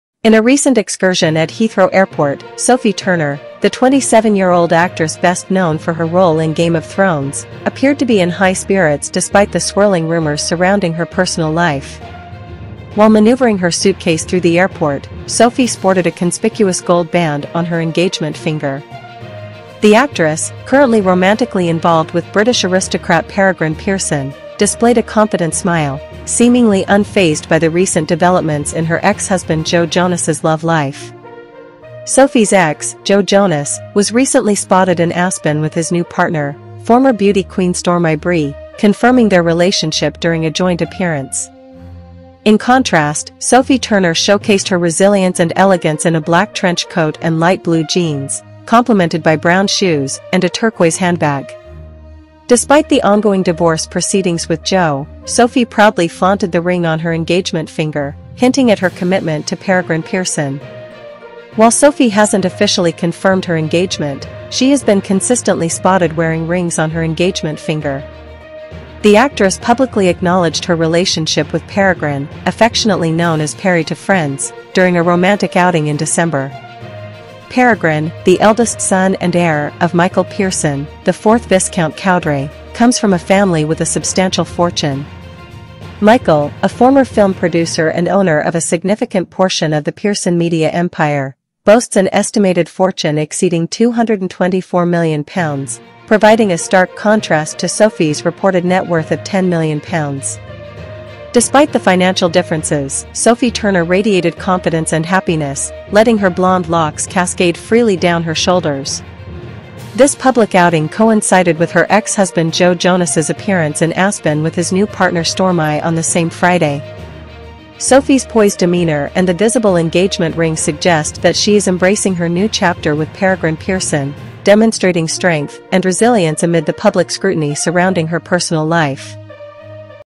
Sophie Turner Sparks Romance Speculation with Peregrine Pearson |Joe Jonas Moves On with Stormi Bree
Sophie Turner, known for her role in Game of Thrones, is making headlines again as she steps out with her new flame, British aristocrat Peregrine Pearson. Despite the recent confirmation of her ex, Joe Jonas, and his new partner Stormi Bree, Sophie appears to be in high spirits during an excursion at Heathrow Airport. The 27-year-old actress, who was previously married to Joe, was spotted wearing a gold band on her engagement finger, fueling speculation about her relationship with Peregrine Pearson.

In this video, we delve into Sophie's recent outing, her fashionable appearance in a black trench coat and light blue jeans, and her seemingly unaffected demeanor amid her ex-husband's publicized new relationship. Sophie's engagement status remains unconfirmed, but she has been spotted multiple times with rings on her engagement finger, and in December, she publicly acknowledged her relationship with Peregrine, affectionately known as Perry to friends.

Peregrine Pearson, the eldest son and heir of Michael Pearson, the 4th Viscount Cowdray, comes from a wealthy family background, with an estimated family fortune exceeding £224 million. In contrast, Sophie Turner's reported net worth is £10 million, making the financial dynamics of their relationship an interesting aspect to explore.